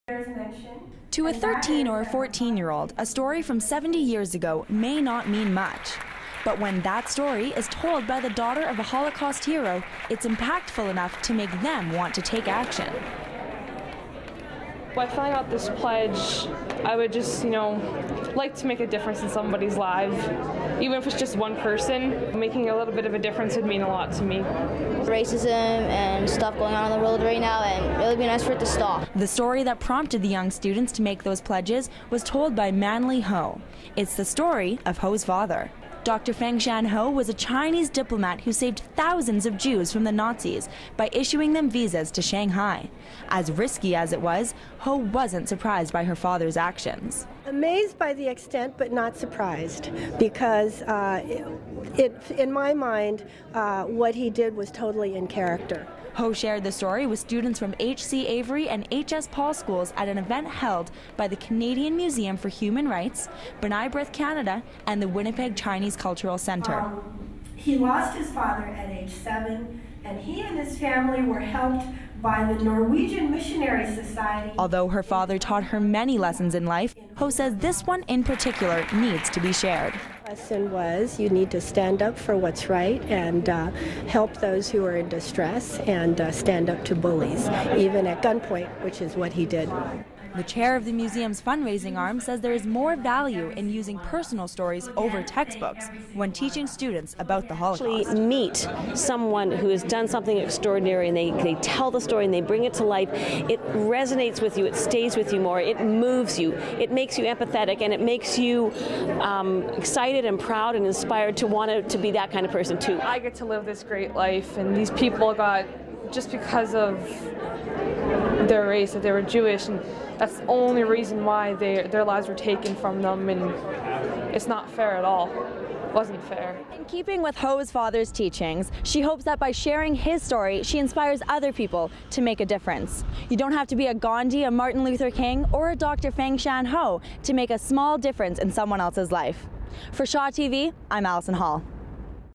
Manli-ho Take Action Event
Shaw TV intern Alison Hall, brings us a story about Manli-ho, daughter of a holocaust hero.